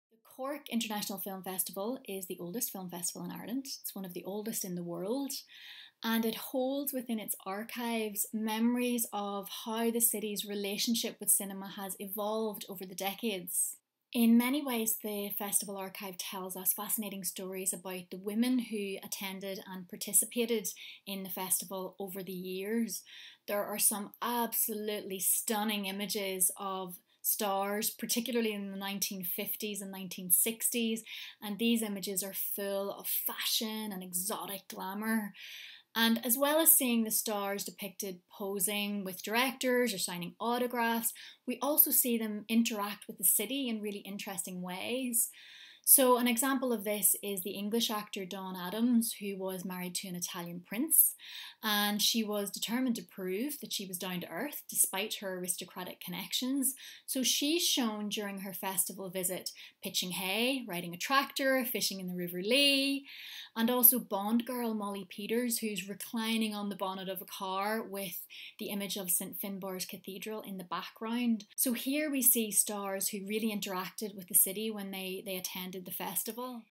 Dr Ciara Chambers discussing the Cork Film Festival's digital archive and "women and the archive"
The digital archive of the Cork International Film Festival is an open access repository of materials from one of the oldest film festivals in the world. Film festivals are markers of the cultural heritage of a place, so this digital resource represents an opportunity to explore the social fabric and cultural soul of Cork city and county, Ireland, and Europe.

This video features Dr Ciara Chambers, head of Film & Screen Media at UCC, who curated the project's first exhibit, "Women and the Archive":

This project represents a collaboration between students and staff within the Department of Digital Humanities and Department of Film and Screen Media at University College Cork, and the Cork International Film Festival. It is ongoing, and further content, descriptions and corrections will be added as this resource continues to develop.